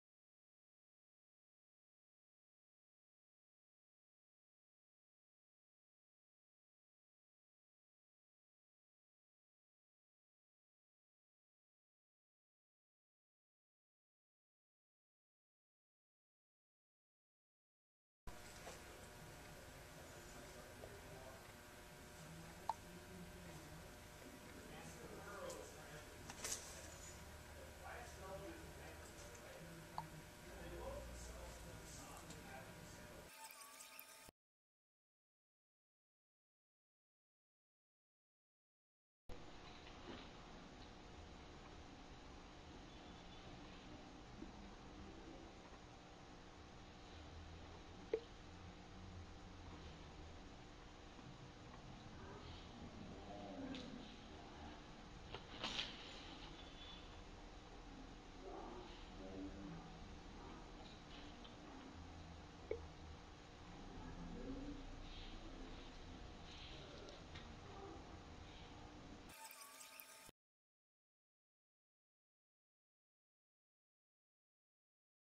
Time by Mariah Thompson (Act 2) - Graphing Stories
Mariah Thompson submits a video of a person holding a clock and watching the time go by. What does the graphing story look like when you compare the time on the clock and the time on the timer?